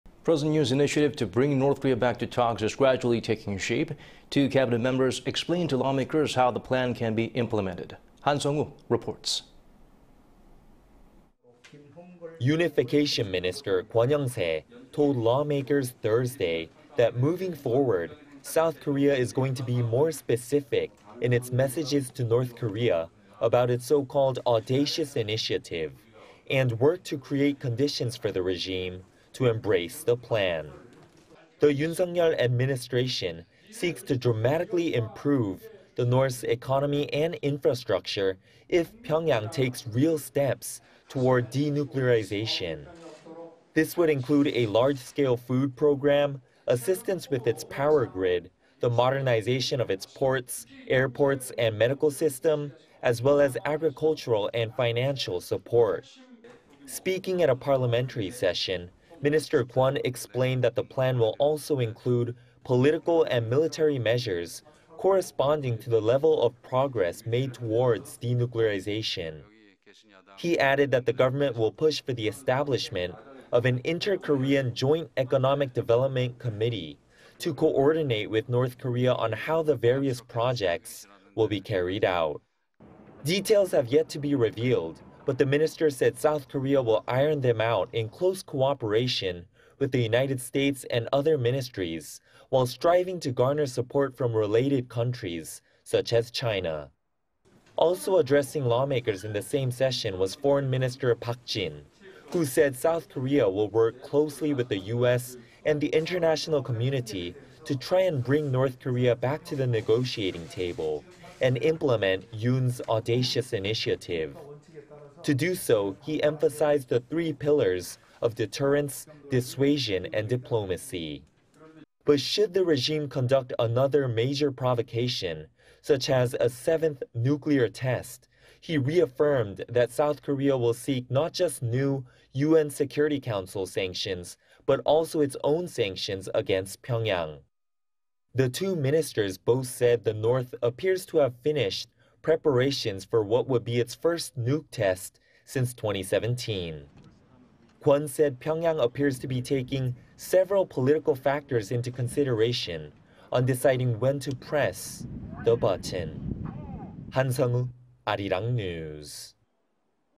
S.Korea to send more 'specific' messages to N.Korea; create conditions for 'audacious initiative'...
권영세 "담대한 구상, 구체적 대북메시지 발신할것"...박진 "미국 등 주요국과 긴밀 협의 추진"

President Yoon's initiative to bring North Korea back to talks... is gradually taking shape.
Two key cabinet members explained to lawmakers how the plan can be implemented.
Han Seong-woo reports.
Unification Minister Kwon Young-se told lawmakers Thursday that moving forward, South Korea is going to be more specific in its messages to North Korea about its so-called "audacious initiative," and work to create conditions for the regime to embrace the plan.
The Yoon Suk-yeol Administration seeks to dramatically improve the North's economy and infrastructure if Pyeongyang takes real steps toward denuclearization.
This would include a large-scale food program, assistance with its power grid, the modernization of its ports, airports and medical system... as well as agricultural and financial support.
Speaking at a parliamentary session, Minister Kwon explained that the  plan will also include political and military measures corresponding to the level of progress made towards denuclearization.
He added that the government will push for the establishment of an Inter-Korean Joint Economic Development Committee to coordinate with North Korea on how the various projects will be carried out.
Details have yet to be revealed, but the Minister said South Korea will iron them out in close cooperation with the United States and other ministries while striving to garner support from related countries such as China.
Also addressing lawmakers in the same session was Foreign Minister Park Jin, who said South Korea will work closely with the U.S. and the international community to try and bring North Korea back to the negotiating table and implement Yoon's "audacious initiative."
To do so, he emphasized the three pillars of deterrence, dissuasion and diplomacy. But should the regime conduct another major provocation,... such as a seventh nuclear test, he reaffirmed that South Korea will seek not just new U.N. Security Council sanctions but also its own sanctions against Pyeongyang.
The two ministers both said the North appears to have finished preparations for what would be its first nuke test since 2017.
Kwon said Pyeongyang appears to be taking several political factors into consideration on deciding when to "press the button."
Han Seong-woo, Arirang News.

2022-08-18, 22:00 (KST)